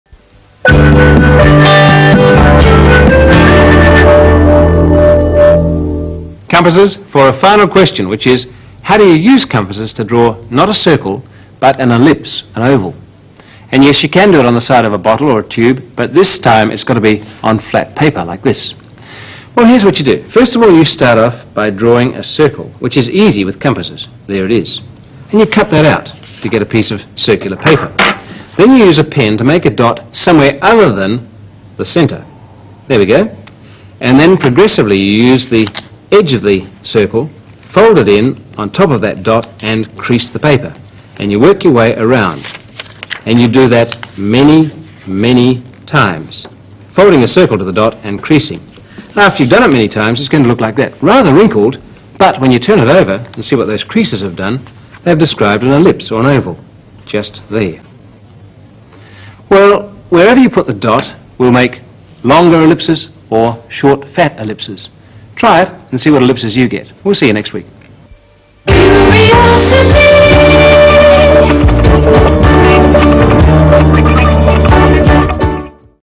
Drawing an ellipse with compasses
Drawing a circle with compasses is easy - that's what they are for - but Rob shows how to draw an ellipse with compasses (and scissors).